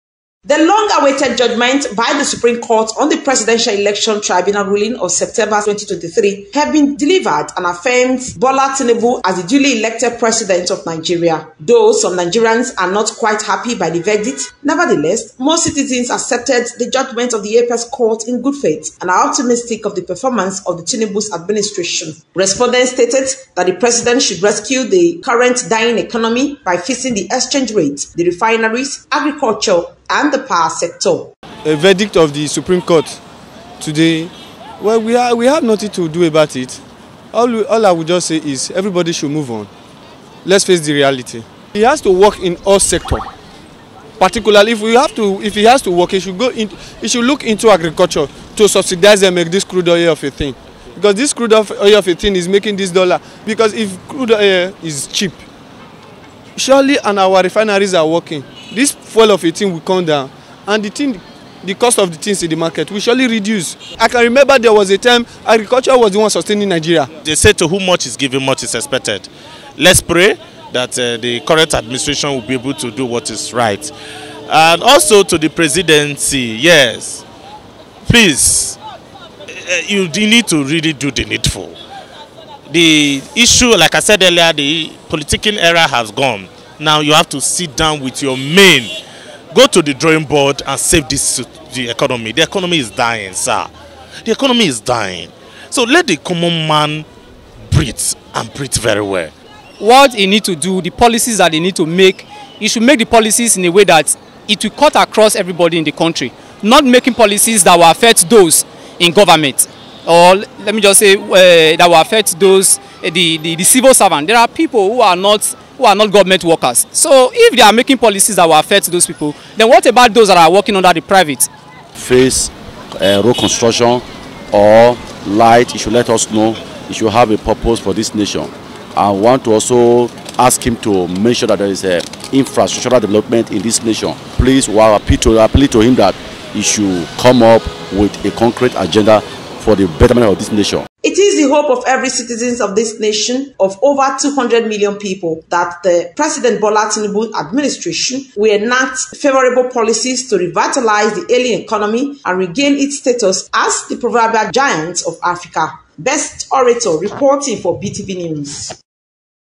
JUDGMENT: Nigerians Sets Agenda For Pres. Tinubu After Supreme Court Judgment
JUDGMENT:

Nigerians Sets Agenda For Pres. Tinubu After Supreme Court Judgment